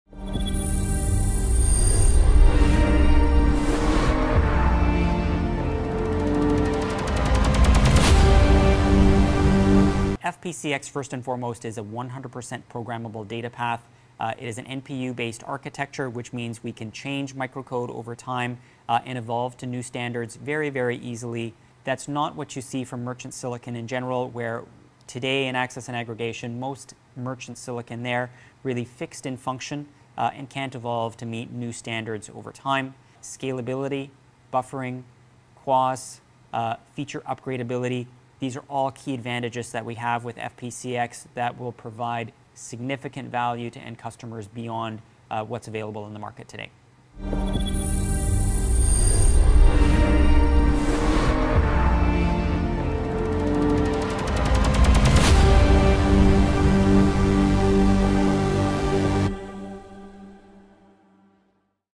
Raise your Xpectations on IP Networking with the new Nokia FPcx network processor
In this video - Jeff Jakab, VP, Hardware Product Management IP Networks, highlights the key benefits of the full programmability features and our “no compromise” design philosophy that underpins our latest network silicon innovation – Nokia FPcx.